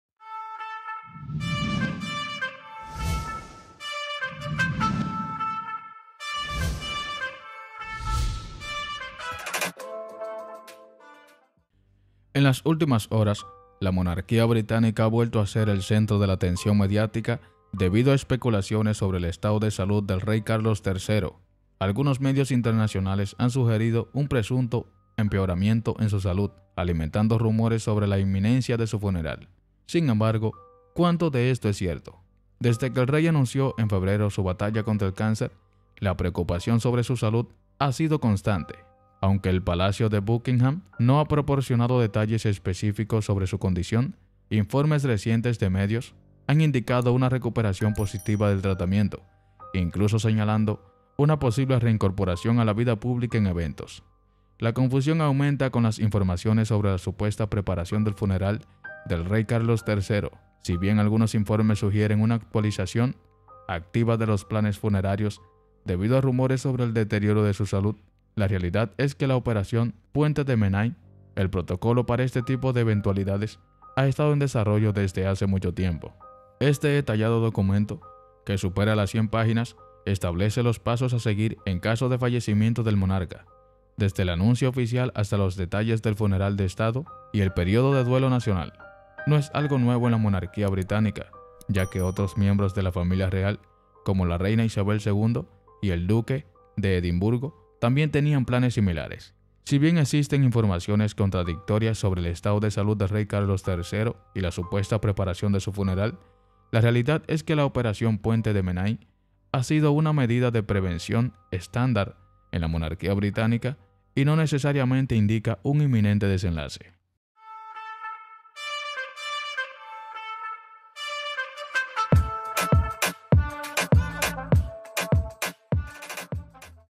La Incógnita del FUNERAL del Rey Carlos III: Lo que No te Cuentan los Medios
Medios internacionales han asegurado en las últimas horas que la salud del monarca habría empeorado y que podría ser "inminente" su funeral

Las informaciones chocan con lo confirmado hasta ahora: analizamos qué hay de cierto en la mencionada preparación de su funeral